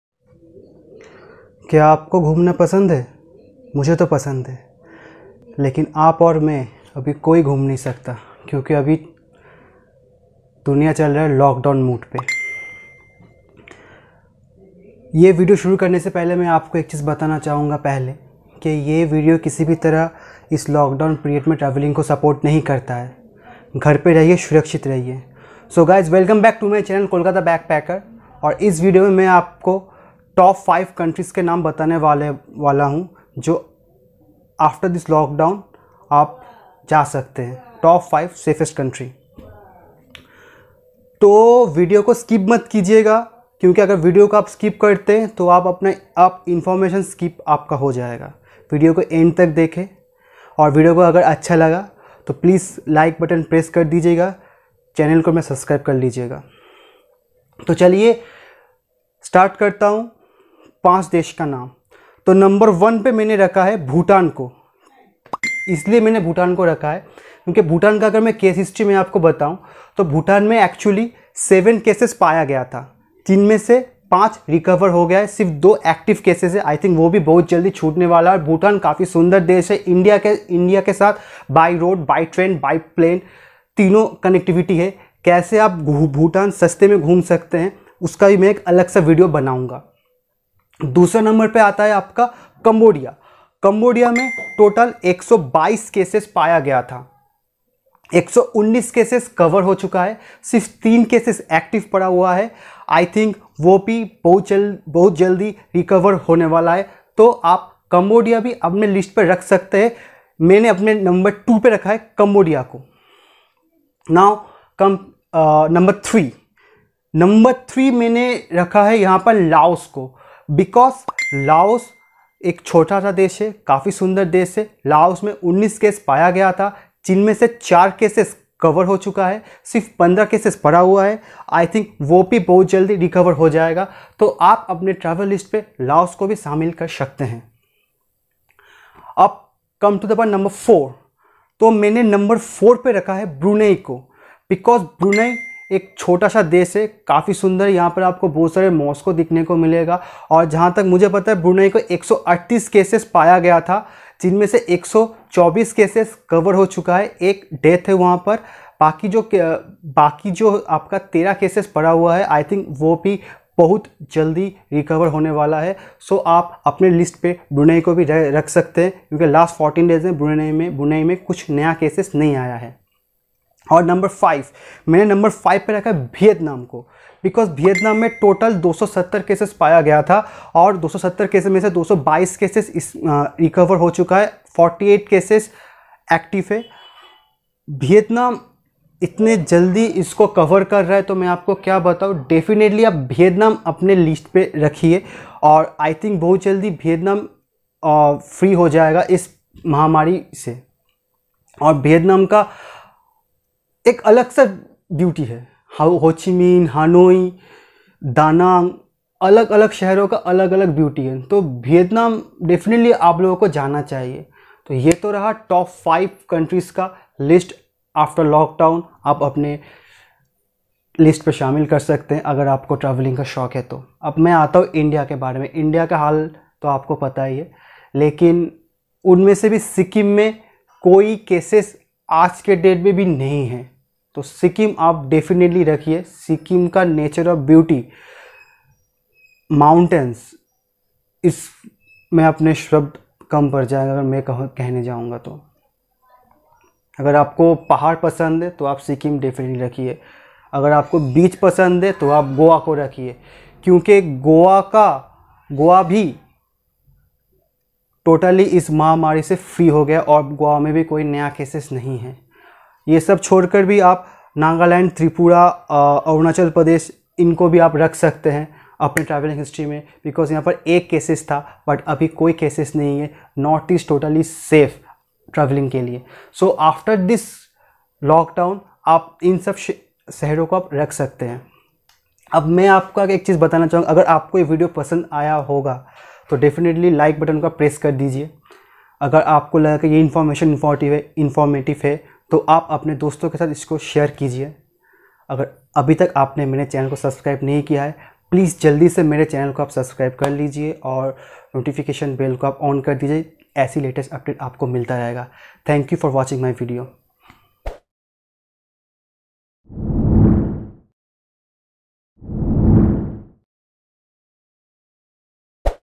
TRAVEL 5 COUNTRIES TO VISIT IN ASIA AFTER LOCKDOWN AND PANDEMIC ENDS
Hope you all are well and safe. In this video you can travel 5 countries to visit in Asia after lockdown and pandemic ends.
Also in this video that I shared information about where to travel in India after lockdown...

All details are mentioned in this video.

Please watch this video full because this video is totally informative. 😊

KEYWORDS